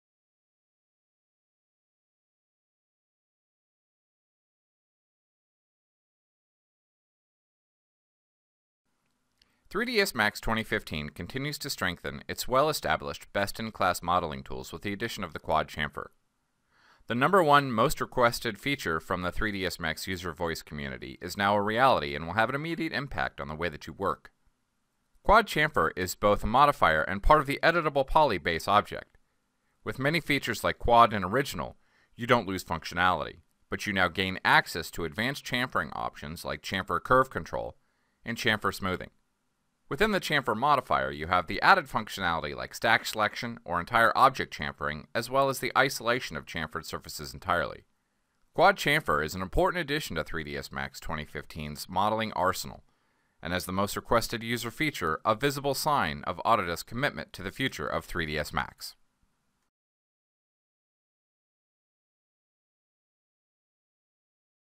Autodesk 3ds Max Design - Chaflán de cuadrícula
Puede crear un borde biselado o achaflanado entre dos superficies compuestas por cuadrículas, en lugar de triángulos, para ayudar a eliminar los pinzamientos.